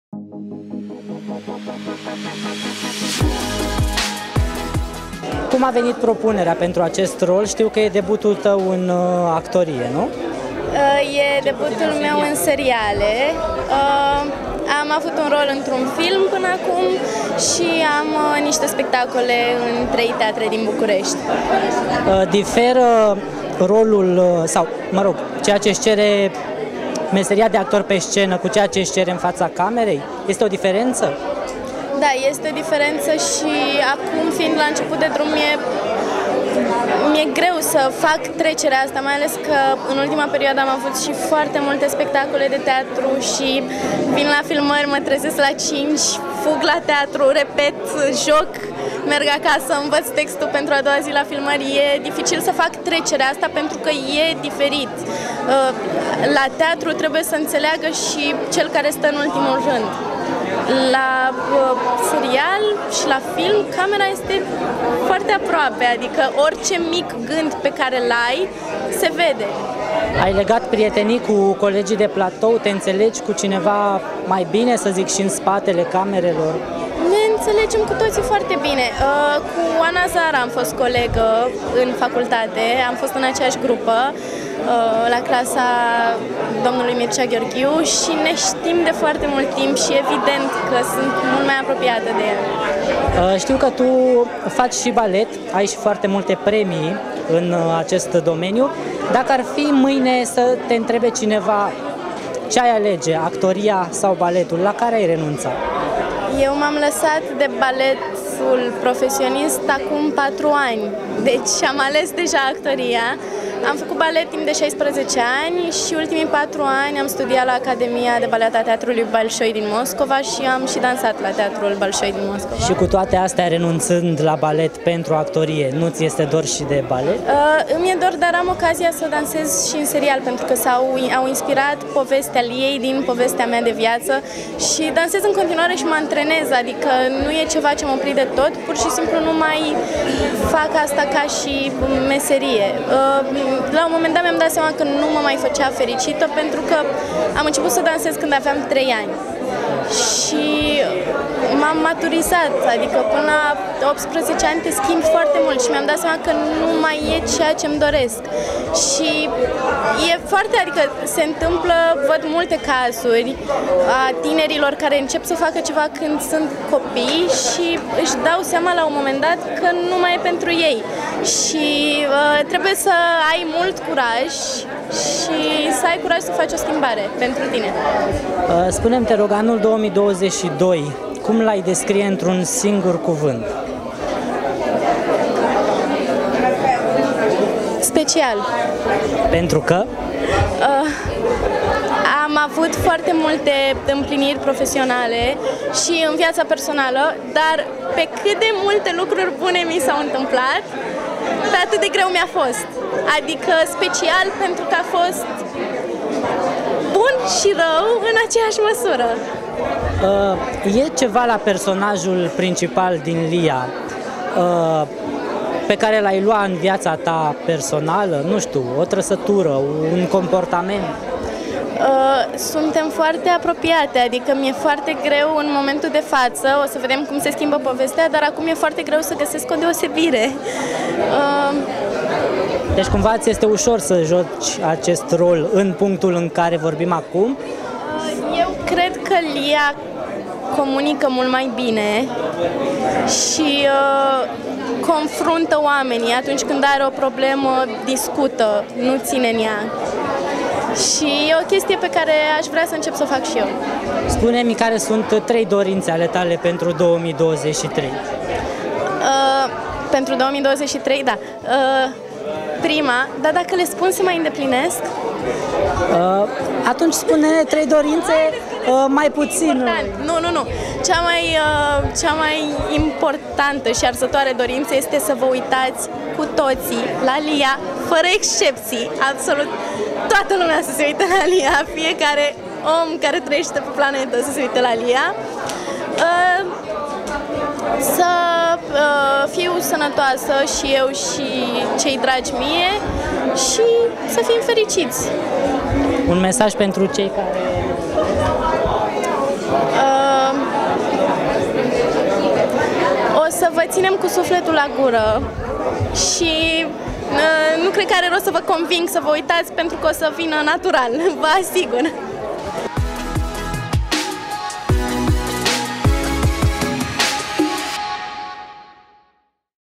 Ana Bodea a renunțat la balet, pentru actorie. "Lia- Soția soțului meu", inspirat din viața ei
Ana Bodea (20 de ani) debutează în actorie, prin rolul principal pe care îl va juca în serialul „Lia- soția soțului meu”, care va fi difuzat pe Antena 1, începând din luna ianuarie.